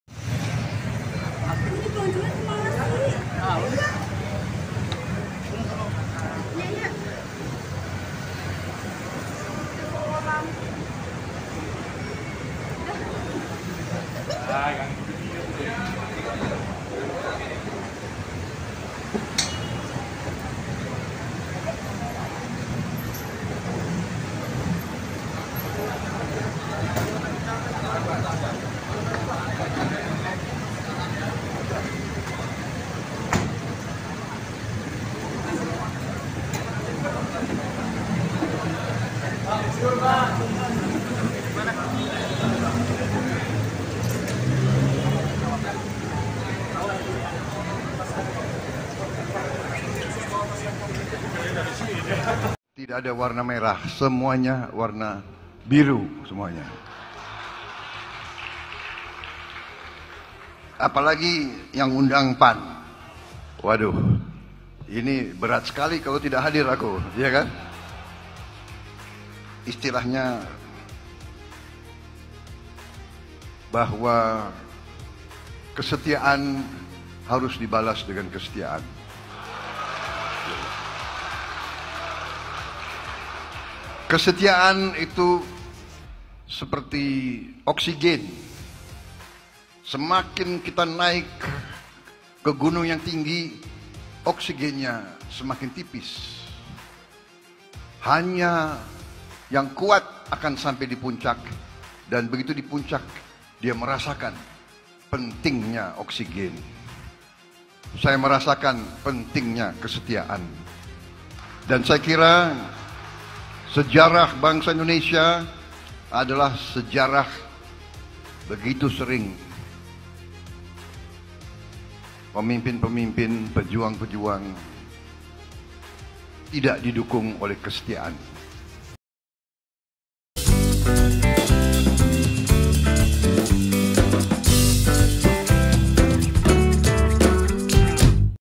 BPK.PRABOWO SUBIANTO HADIRI RAKORNAS PAN-PARTAI AMANAH NASIONAL
calon presiden  terpilih,prabowo subianto hadiri acara workshop bimtek anggota legislatif tingkat nasional partai amanah nasional (PAN)dan rakornas pemenangan pilkada serentak 2024 di hotel JS luwansa.kuningan jaksel.